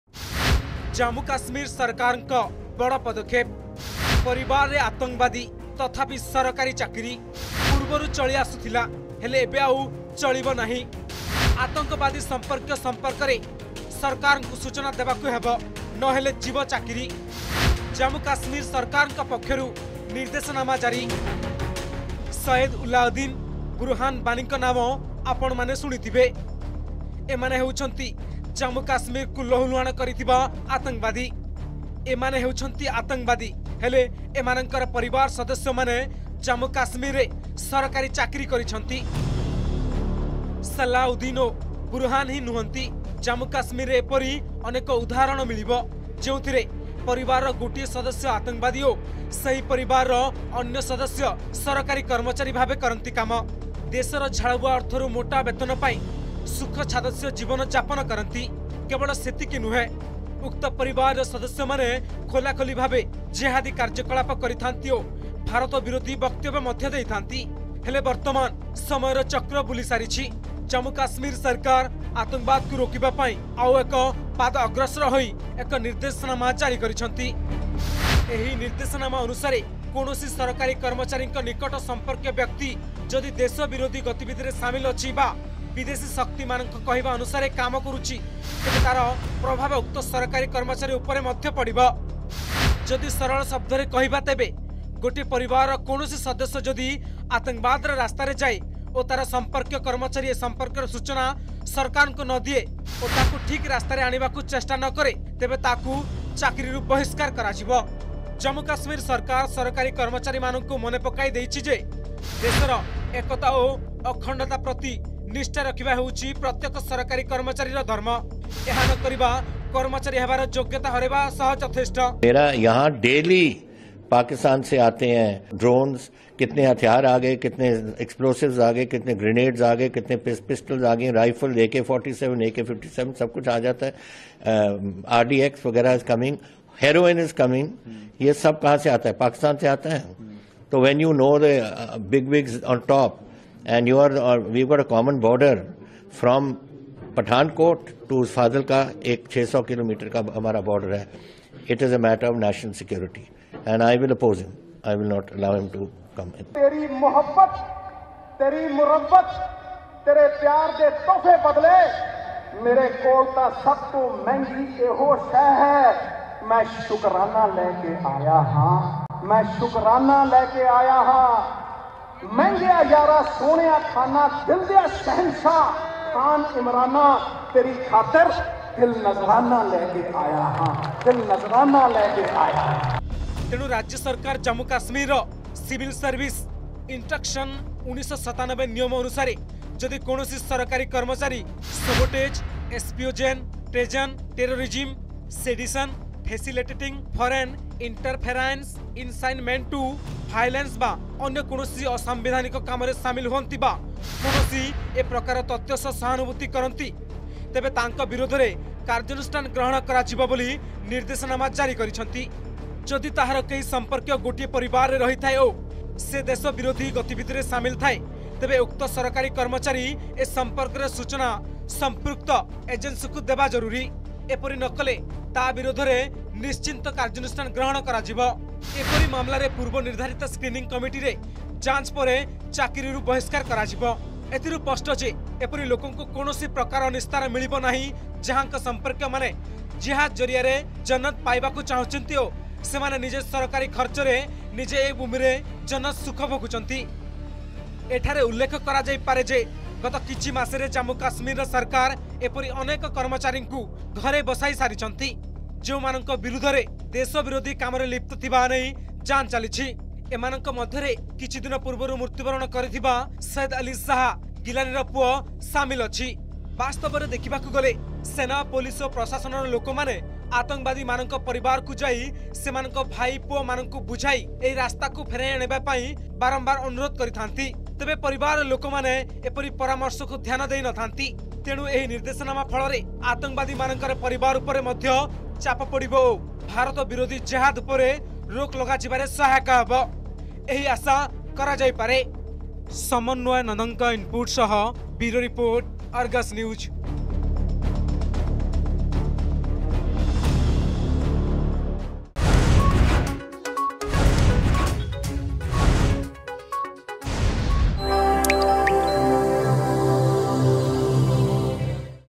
ଜାମ୍ମୁ କାଶ୍ମୀର ସରକାରଙ୍କ ବଡ଼ ପଦକ୍ଷେପ
ଜାମ୍ମୁ କାଶ୍ମୀର ସରକାର ଆତଙ୍କବାଦକୁ ରୋକିବା ପାଇଁ ଆଉ ଏକ ପାଦ ଅଗ୍ରସର ହୋଇ ଏକ ନିର୍ଦେଶନାମା ଜାରୀ କରିଛନ୍ତି । ଏହି ନିର୍ଦେଶନାମା ଅନୁସାରେ କୌଣସି ସରକାରୀ କର୍ମଚାରୀଙ୍କର ନିକଟ ସମ୍ପର୍କିୟ ବ୍ୟକ୍ତି ଯଦି ଦେଶ ବିରୋଧୀ ଗତିବିଧିରେ ସାମିଲ ଅଛି  ବା ବିଦେଶୀ ଶକ୍ତି ମାନଙ୍କ କହିବା ଅନୁସାରେ କାମ କରୁଛି ତେବେ ତାର ପ୍ରଭାବ ଉକ୍ତ ସରକାରୀ କର୍ମଚାରୀ ଉପରେ ମଧ୍ୟ ପଡିବ ।